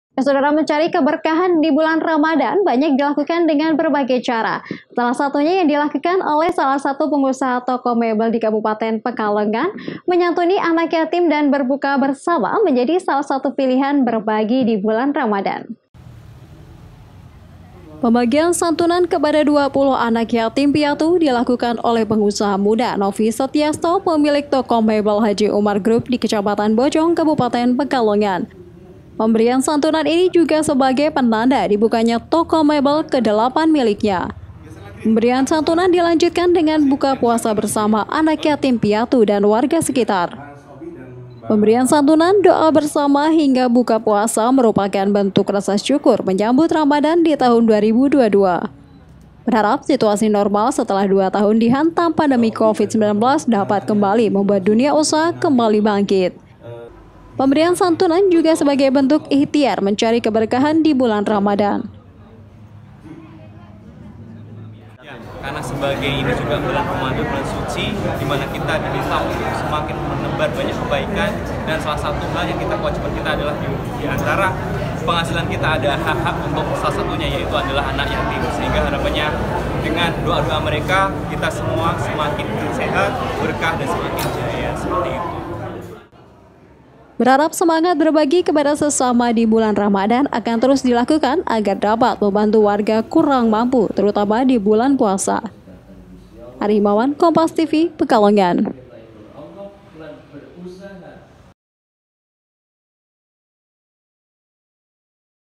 Berbagi di Bulan Ramadhan Santuni Anak yatim
PEKALONGAN, KOMPAS.TV - Pembagian santunan kepada dua puluh anak yatim piatu dilakukan oleh pengusaha muda Novi Setyasto, pemilik toko mebel Haji Umar Grup di Kecamatan Bojong, kabupaten Pekalongan. Pemberian santunan ini juga sebagai penanda dibukanya toko mebel ke delapan miliknya.

Pemberian santunan dilanjutkan dengan buka puasa bersama bersama anak yatim piatu dan warga sekitar. Pemberian santunan, doa bersama hingga buka puasa merupakan bentuk rasa syukur menyambut ramadhan di tahun 2022.

Berharap situasi normal setelah dua tahun dihantam pandemi Covid-19 dapat kembali membuat dunia usaha kembali bangkit. Pemberian santunan juga sebagai bentuk ikhtiar mencari keberkahan di bulan ramadhan.

Berharap semangat berbagi kepada sesama di bulan ramadhan akan terus dilakukan agar dapat membantu warga kurang mampu terutama dibulan puasa.